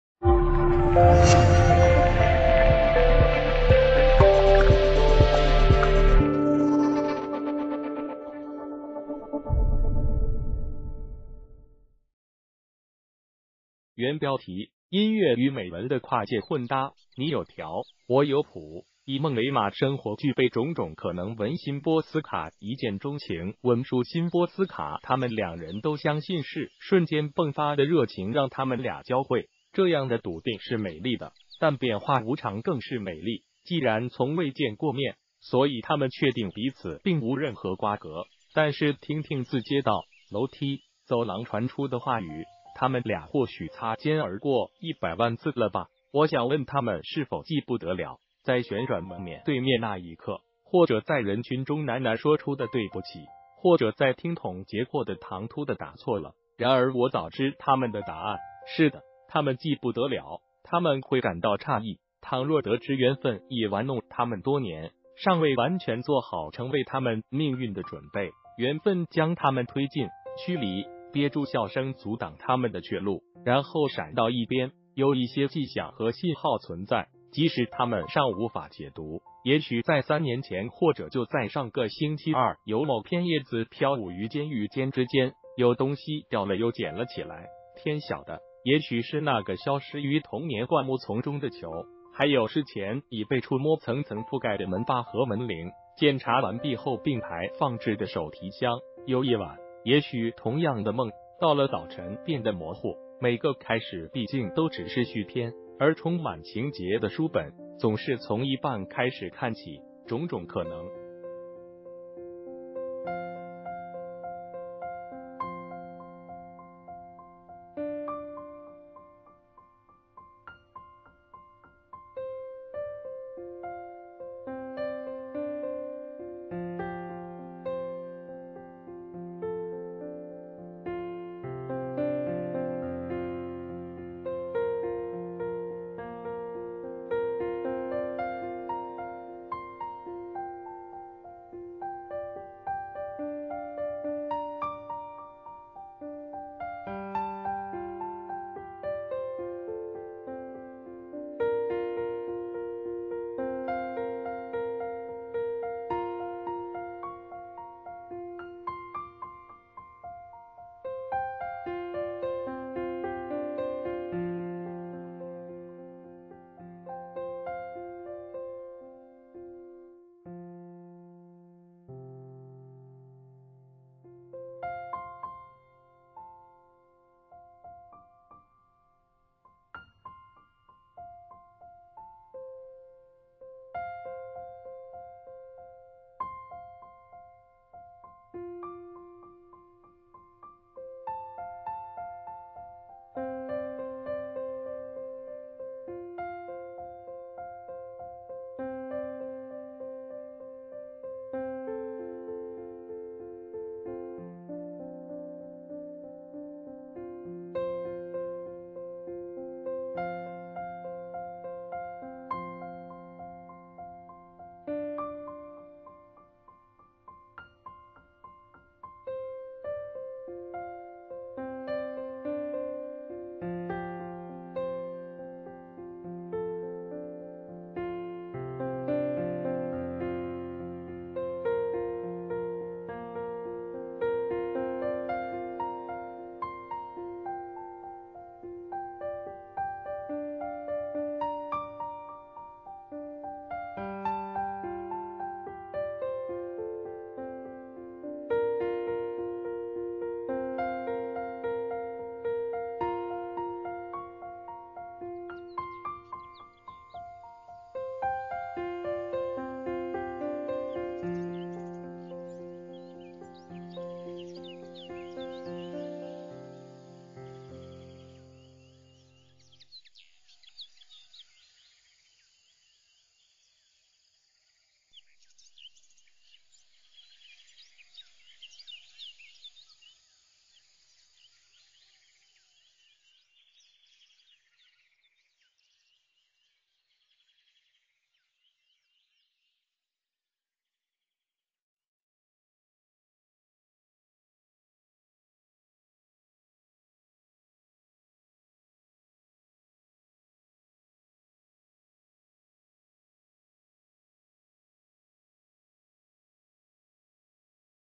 辛波丝卡：以梦为马，生活具备种种可能
谢谢！原标题:▲音乐与美文的跨界混搭，你有调，我有谱。以梦为马生活具备种种可能文|辛波丝卡一见钟情文丨辛波丝卡他们两人都相信是瞬间迸发的热情让他们俩交会。这样的笃定是美丽的，但变化无常更是美丽。既然从未见过面，所以他们确定彼此并无任何瓜葛。但是听听自街道、楼梯、走廊传出的话语——他们俩或许擦肩而过一百万次了吧？我想问他们是否记不得了——在旋转门面对面那一刻？或者在人群中喃喃说出的“对不起”？或者在听筒截获的唐突的“打错了”？然而我早知他们的答案。是的，他们记不得了。他们会感到诧异，倘若得知缘分已玩弄他们多年。尚未完全做好成为他们命运的准备，缘分将他们推近，驱离，憋住笑声阻挡他们的去路，然后闪到一边。有一些迹象和信号存在，即使他们尚无法解读。也许在三年前或者就在上个星期二有某片叶子飘舞于肩与肩之间？有东西掉了又捡了起来？天晓得，也许是那个消失于童年灌木丛中的球？还有事前已被触摸层层覆盖的门把和门铃。检查完毕后并排放置的手提箱。有一晚，也许同样的梦，到了早晨变得模糊。每个开始毕竟都只是续篇，而充满情节的书本总是从一半开始看起。种种可能